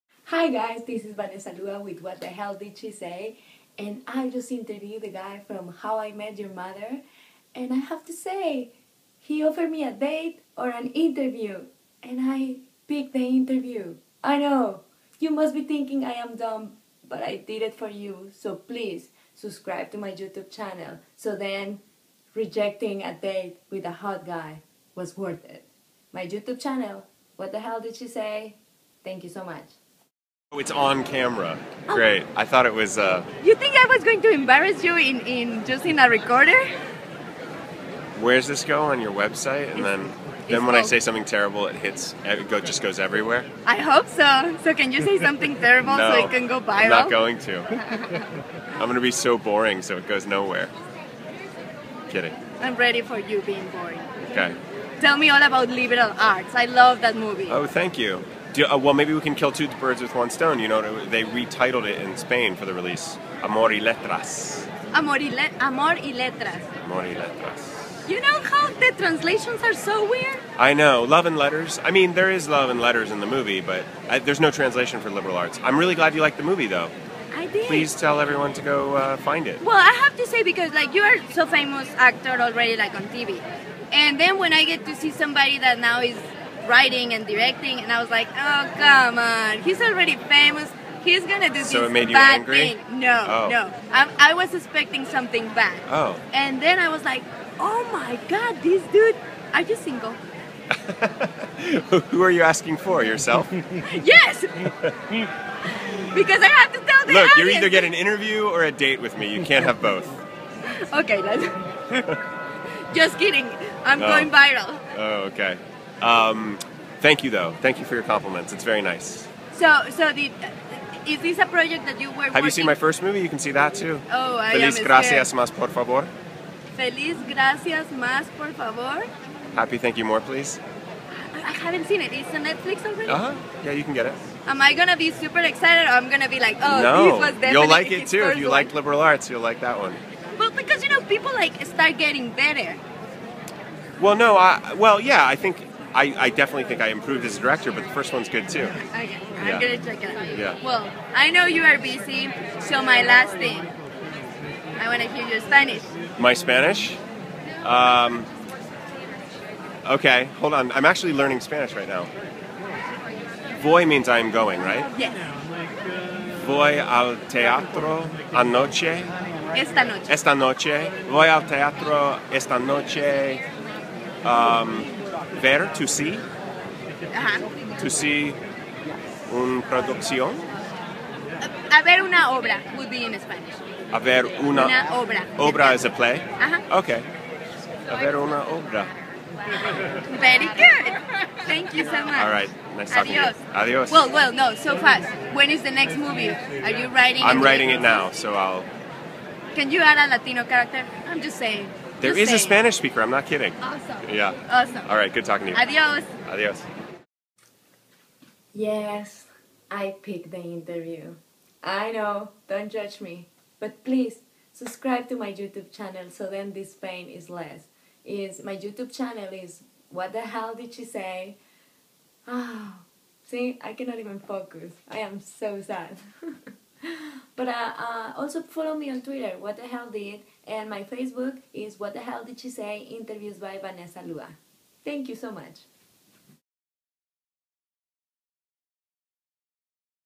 How I Met Your Mother Actor Josh Radnor (Ted Mosby) - Fun Interviews
I interviewed Josh Radnor (Ted Mosby in How I met your mother) the guy is amazing so I asked him if he was single! What do you think he answered?
Let's be in touch!
You guys are awesome. PS: Did you know today (07-29) is Josh Birthday???